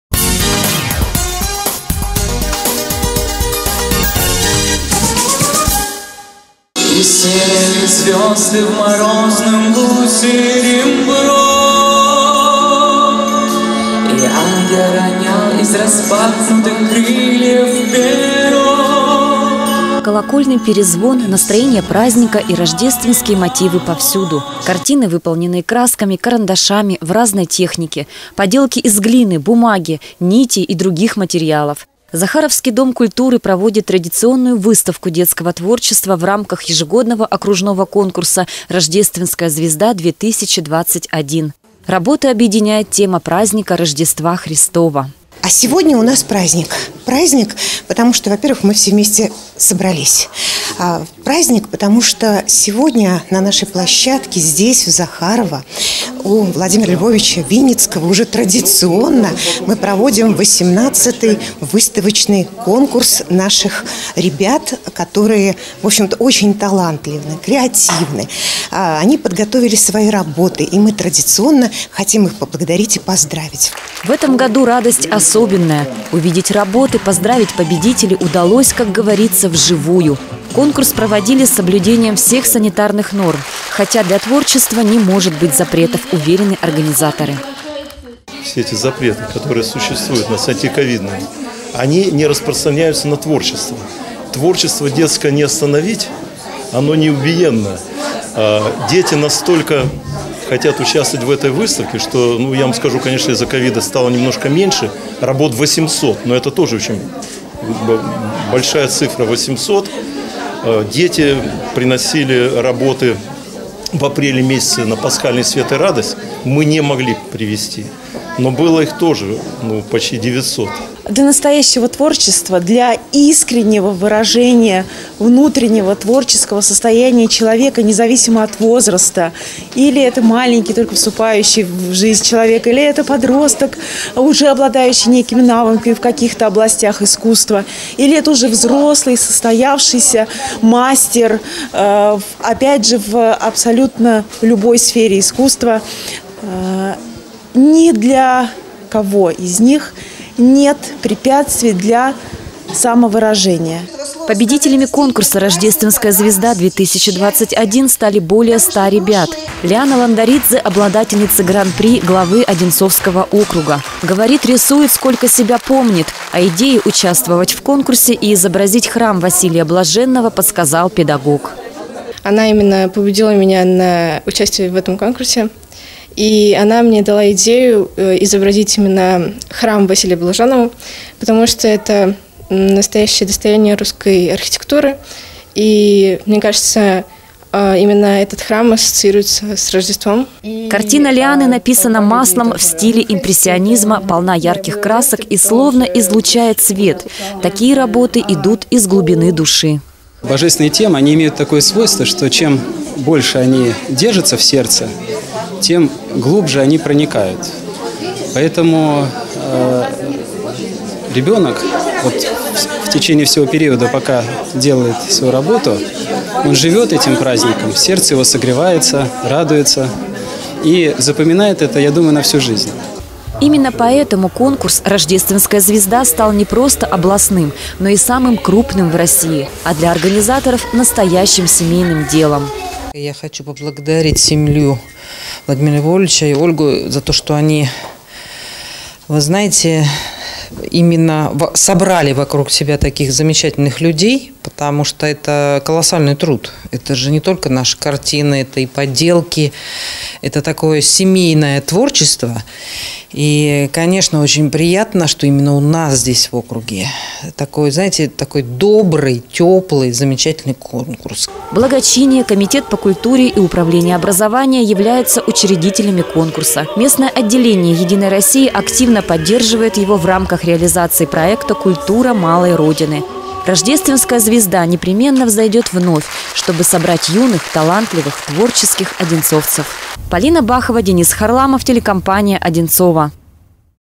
Рождественская звезда
В Захаровском Доме культуры состоялась церемония награждения победителей ежегодного окружного конкурса детско-юношеского рисунка и прикладного искусства «Рождественская звезда — 2021».